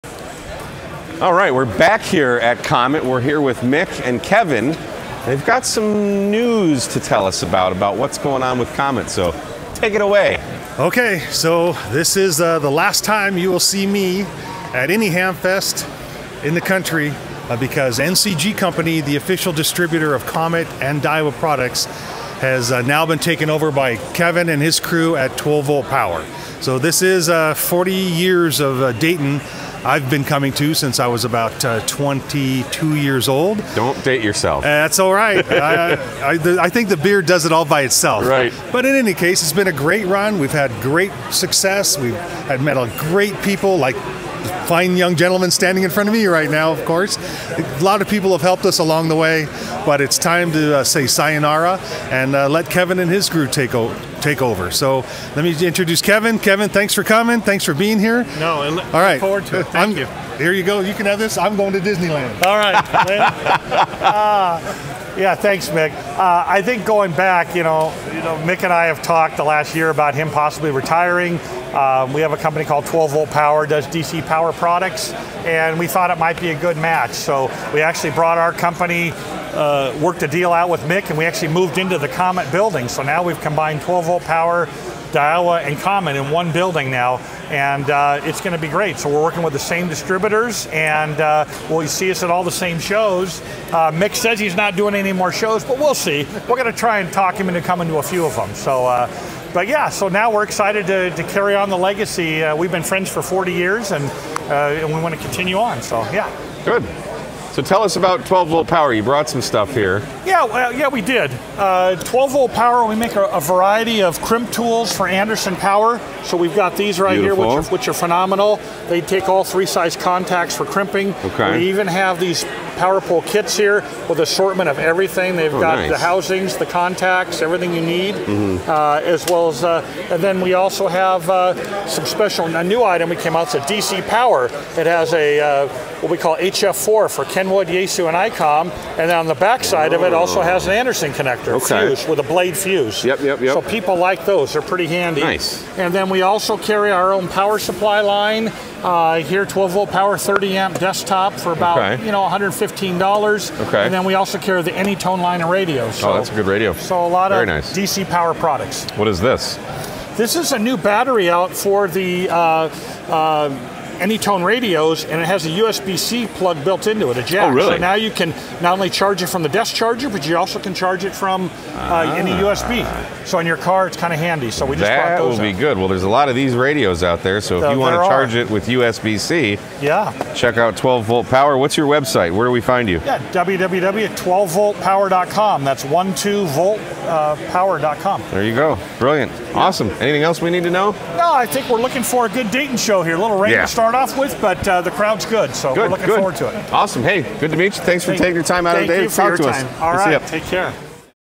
Comet Antennas Is Now With 12Volt Power Hamvention 2024
Mick From Comet Is Retiring & Handing Over The Reigns To The Very Capable 12Volt Power Company.  Congrats On Your Retirement Mick, We'll Miss You!